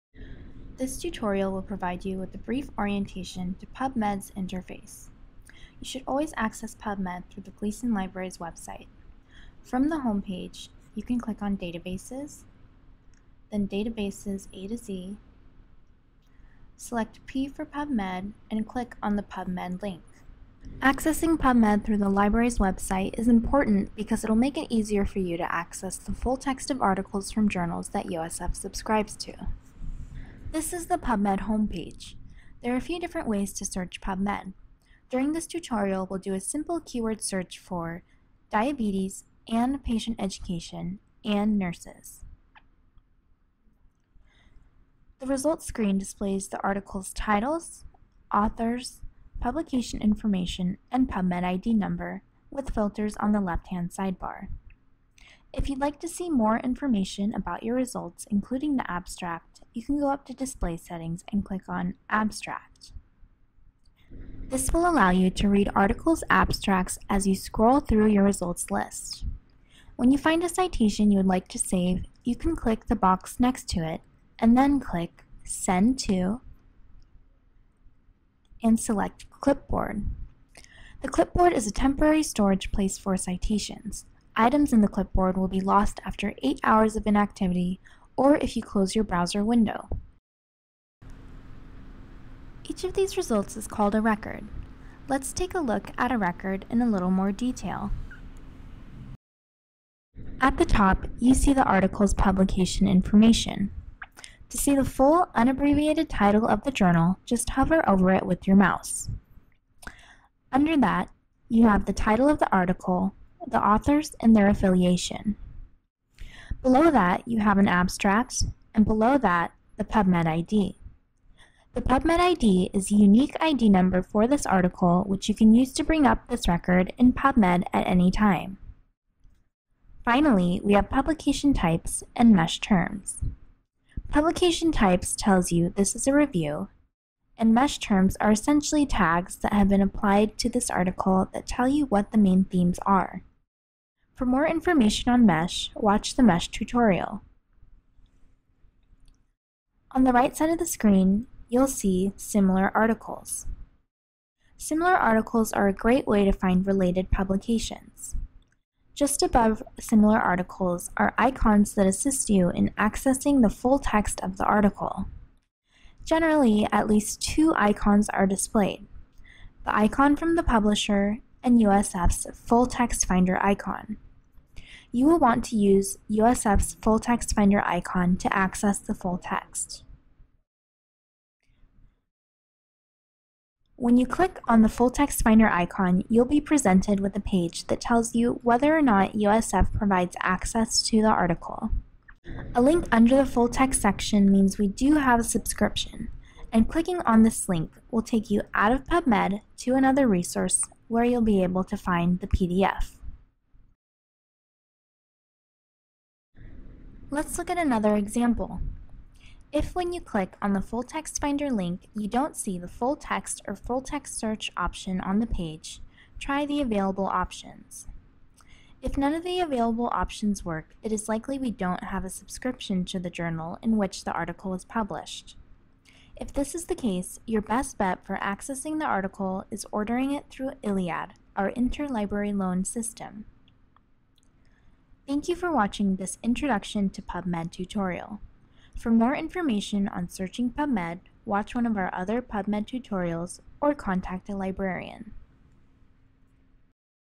Introduction to PubMed
Brief introduction to keyword searching in PubMed.